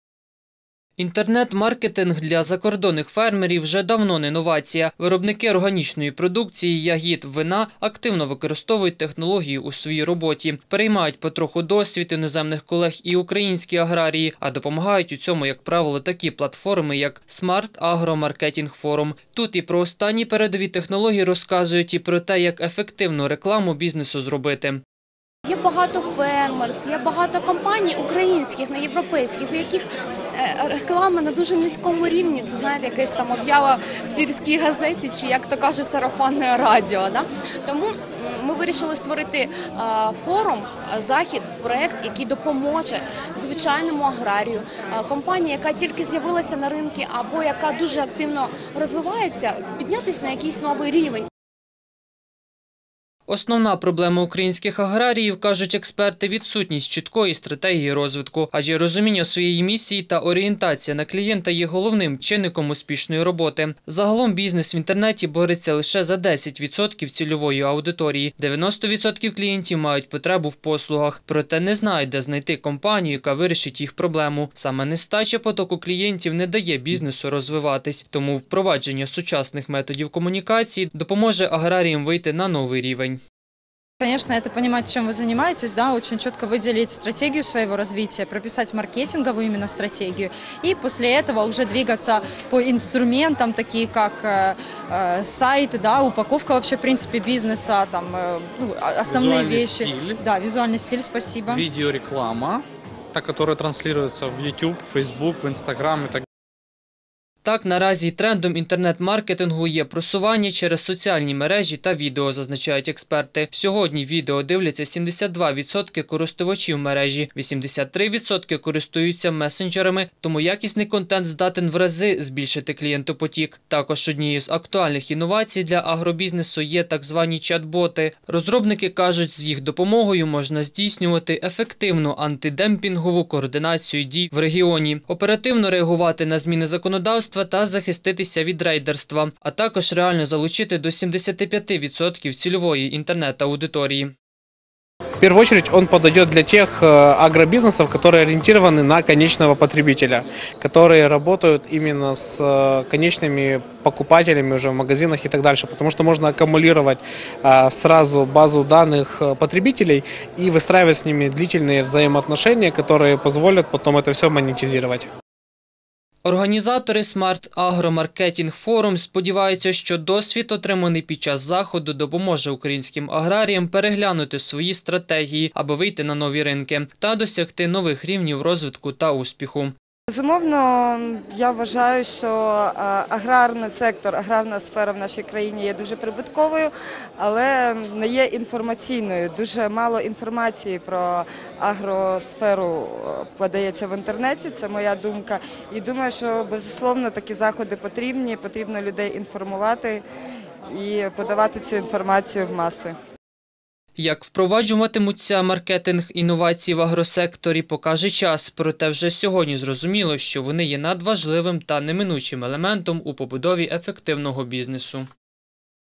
В Киеве прошел Smart Agro Marketing Forum
На платформе Smart Agro Marketing Forum обсуждают новинки технологий и о том, как сделать эффективную рекламу для бизнеса. К примеру, аграрии могут искать клиентуру через интернет, где можна презентовать свои ролики и делать имидж-рекламу фирмы.